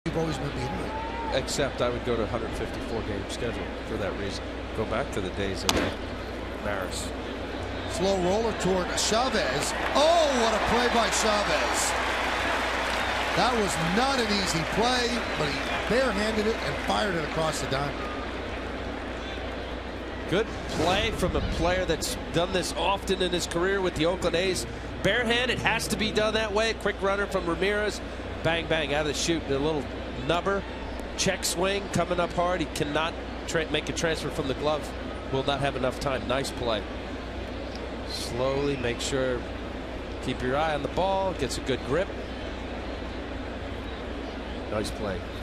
Chavez's barehanded play
CWS@NYY: Chavez makes a nice play with his bare hand